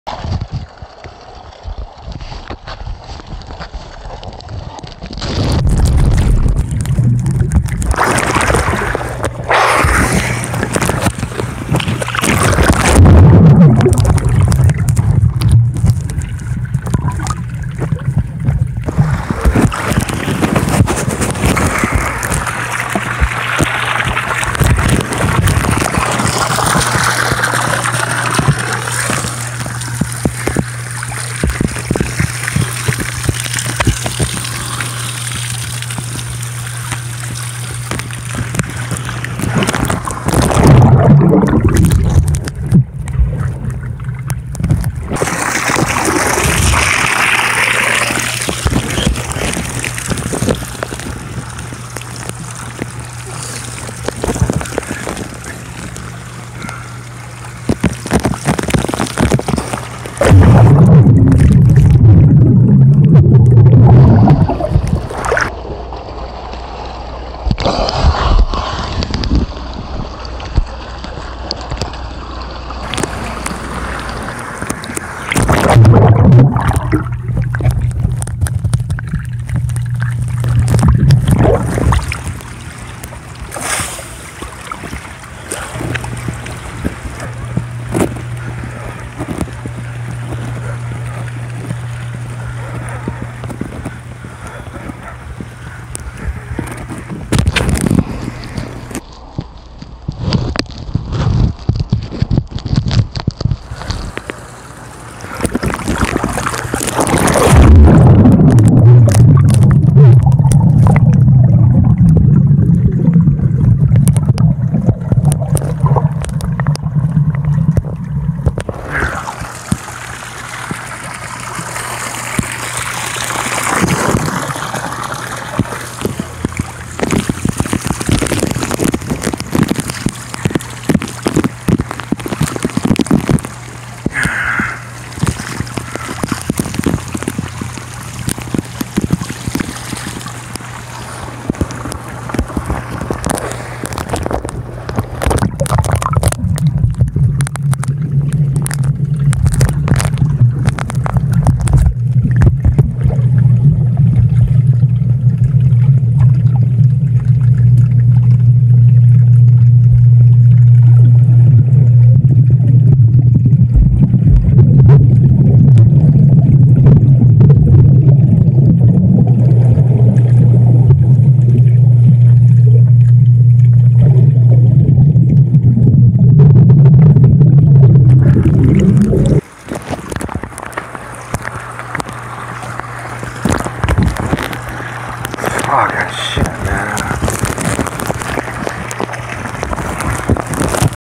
3 11 18 Swim
Paradise Point Cape Coral FL 3 11 18 No music. On the 11th day of being sick with the flu I went swimming.